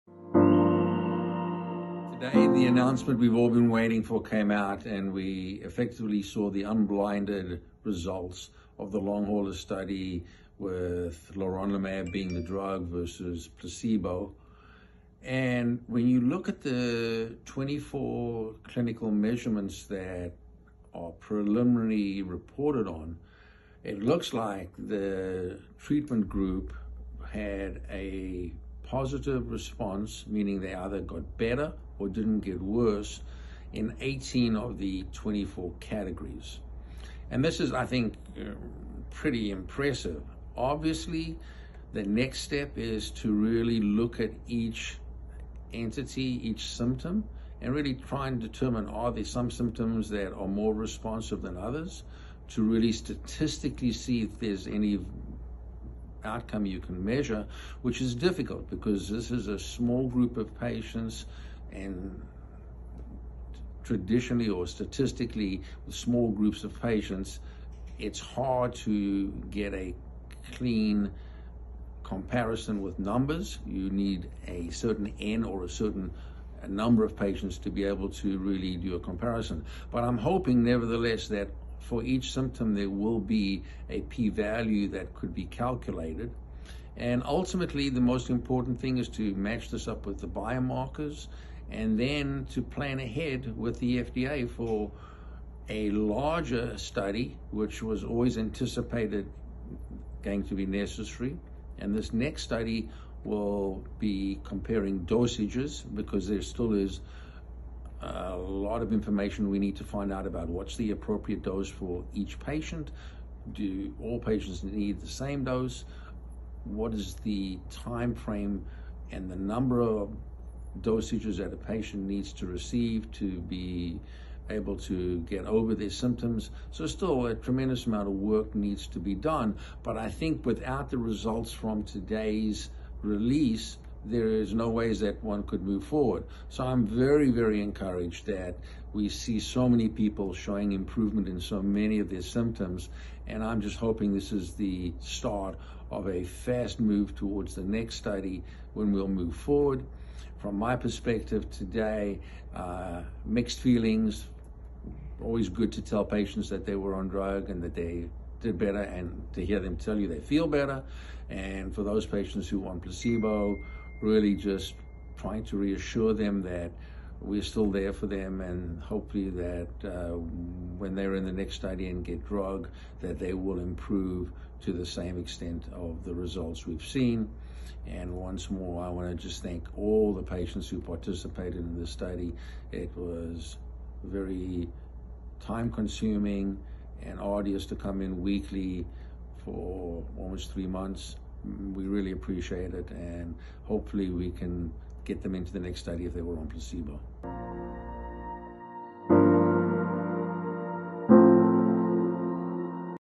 Long Hauler Study Follow-Up
Covid-19 Long Hauler Clinical Trial Preliminary Results Are Released By CytoDyn